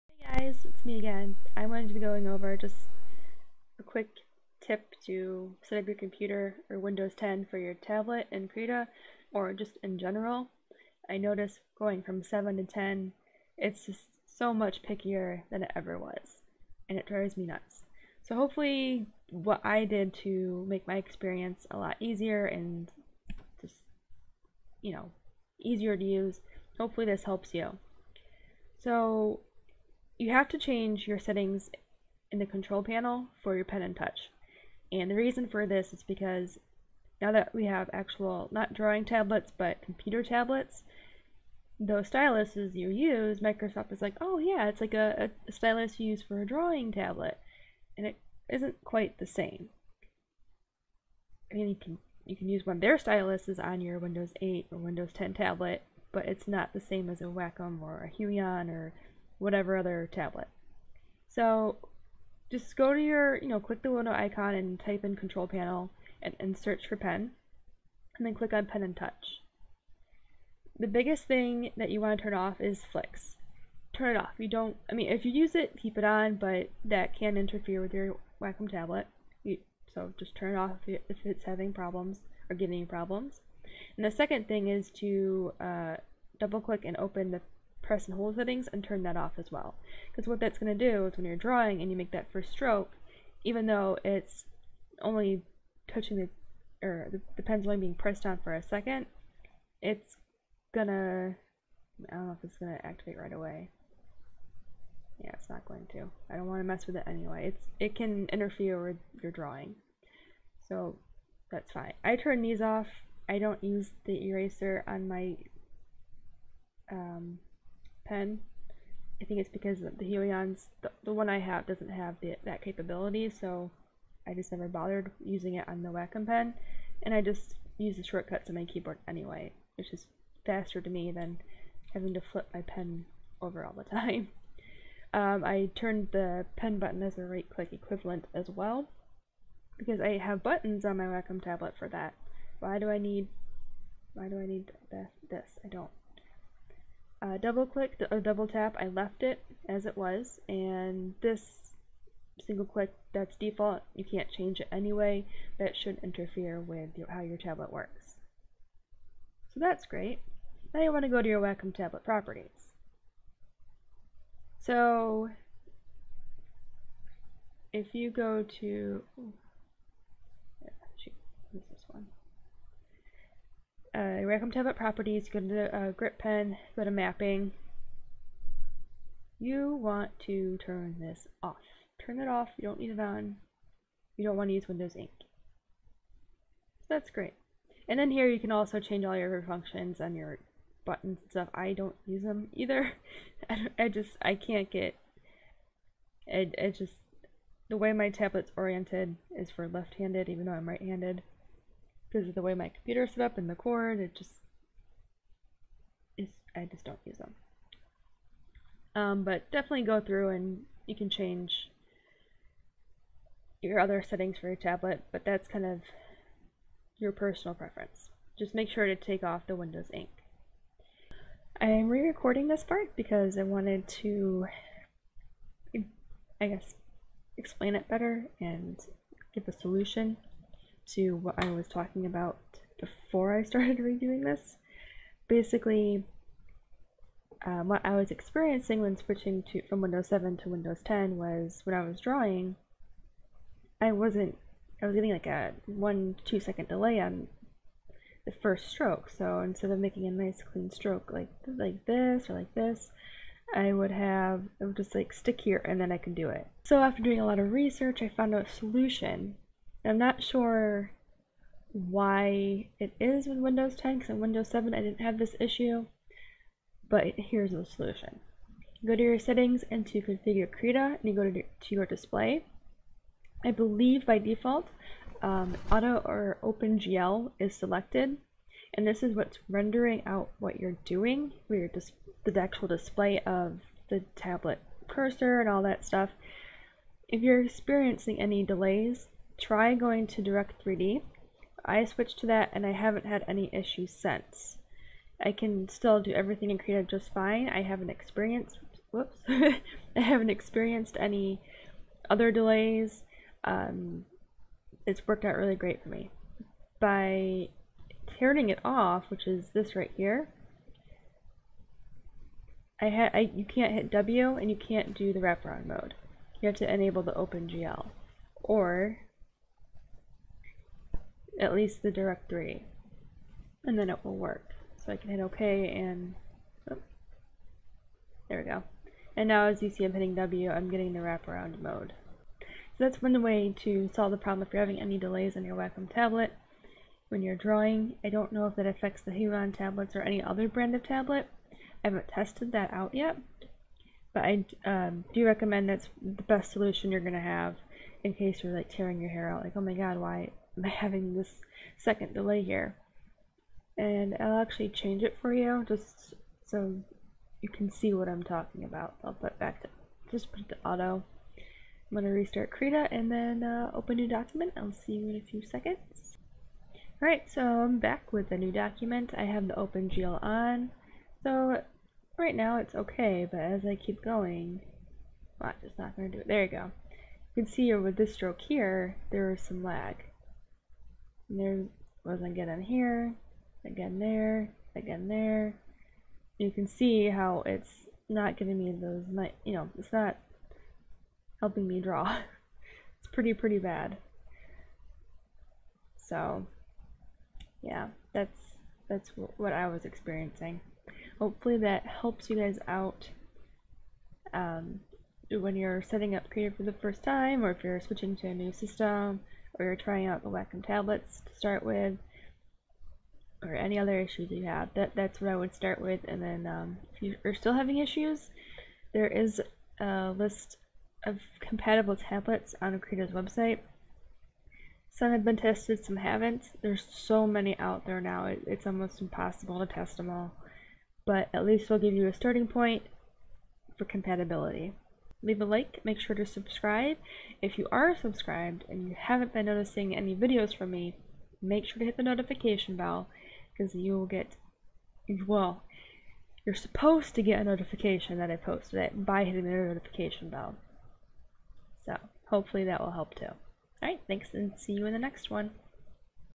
Windows 10, Krita, and Tablet setup
This is always the biggest headache for windows users. All it takes is one update and BOOM everything gets set back to the way it was! Use this video as reference to make sure you never forget what settings to use if you have issues :)

If you want to see what tablets are officially supported or unconfirmed, check out this link: